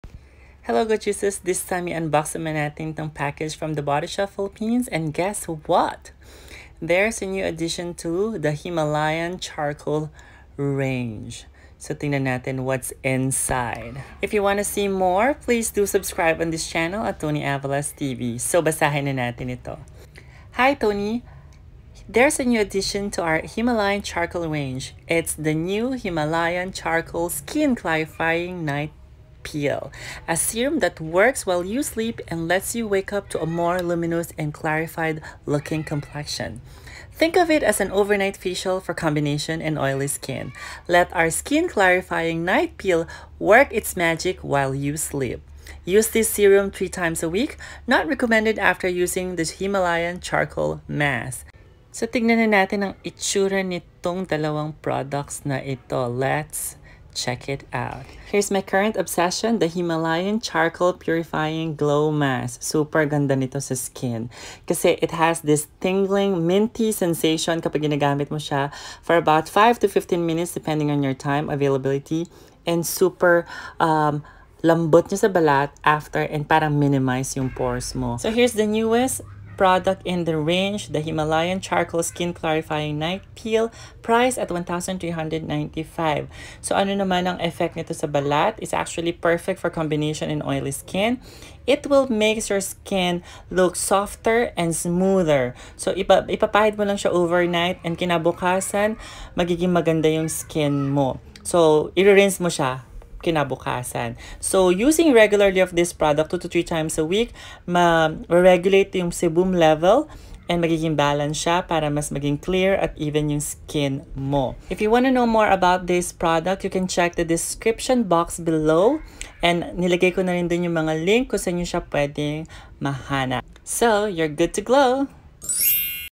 The Body Shop Himalayan Charcoal Purifying Mask and Night Peel | UNBOXING
THE BODY SHOP HIMALAYAN CHARCOAL

Purifying Glow Mask P1,195
- 2-3 x a week
- 5-15 mins on the skin, rinse off
- smoother, purified skin, pores seem to be minimized

Skin Clarifying Night Peel P1,395
- overnight application
- 2-3 x a week
- rinse off in the morning
- skin looks even and smoother

Both perfect for combi and oily skin